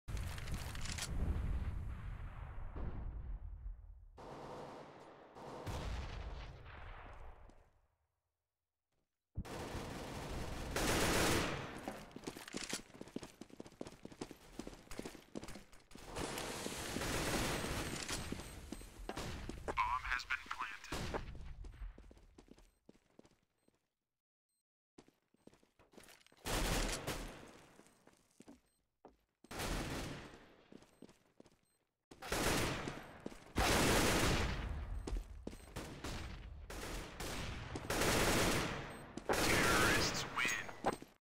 Hiko vs. Frost Gaming - ESEA Invite Season 14
Hiko aces to win the match!